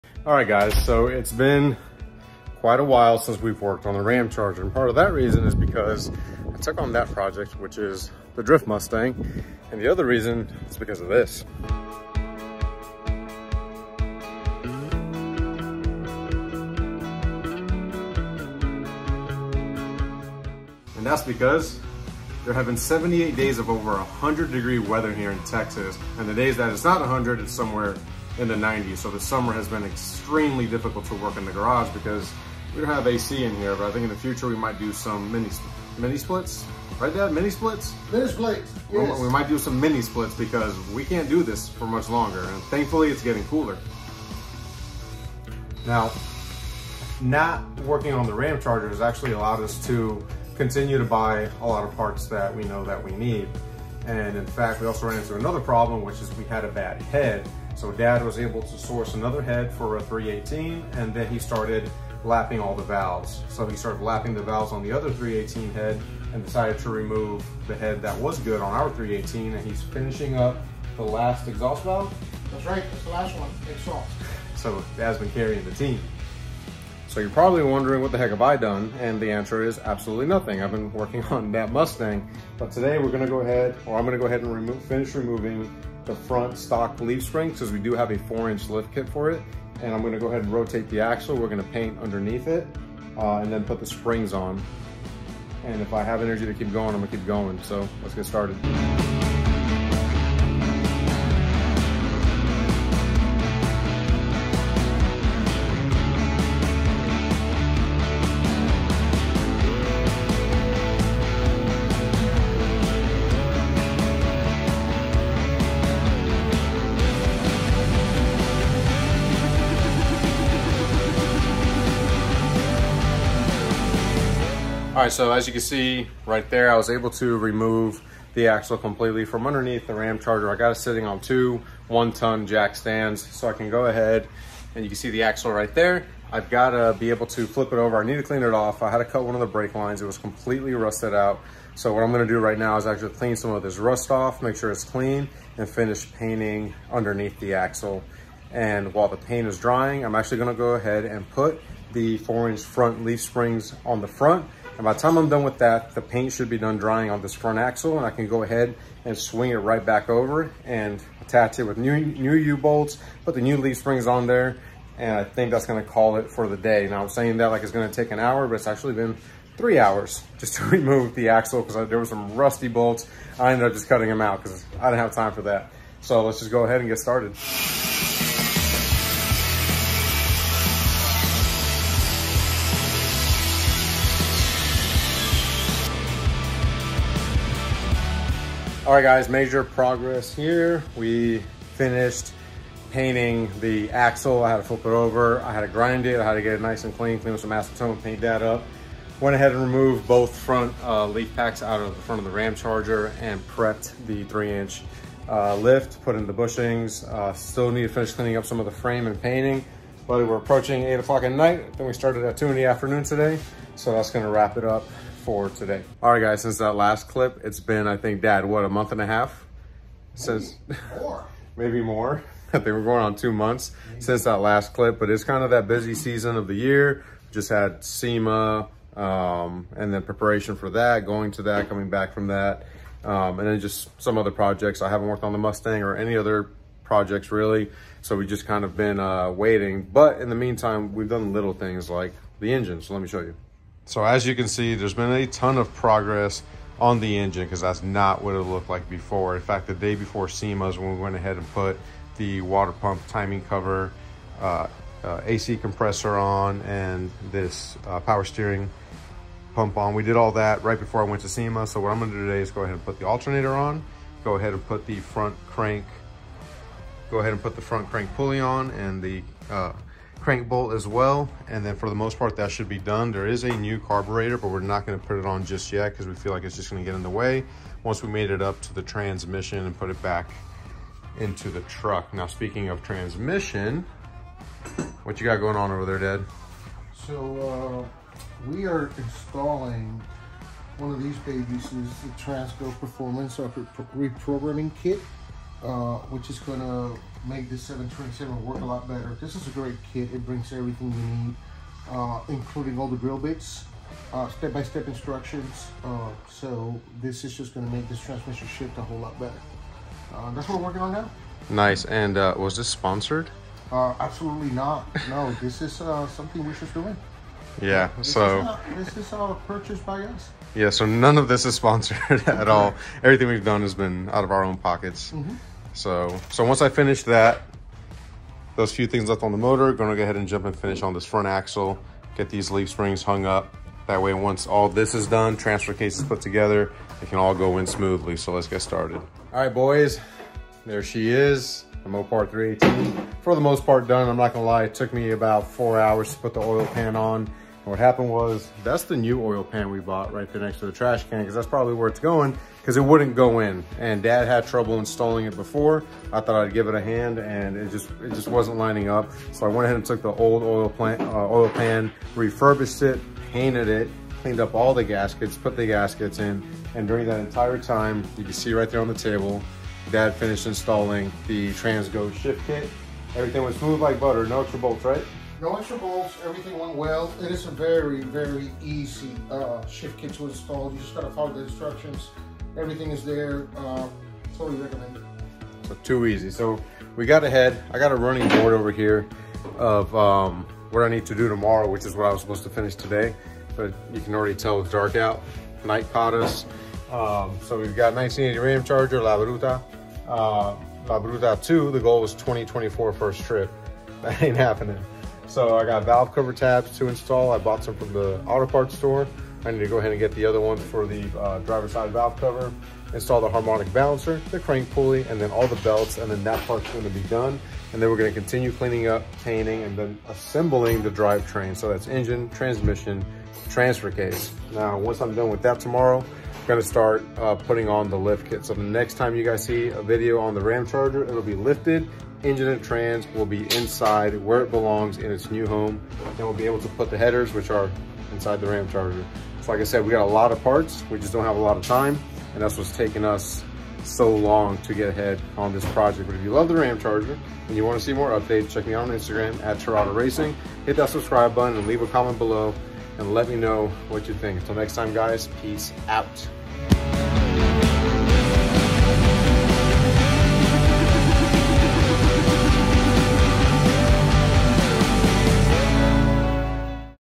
Ramcharger 318 V8 Engine Rebuild and Trans Go Shift Kit!
Bronco Sport
Tools and Hardware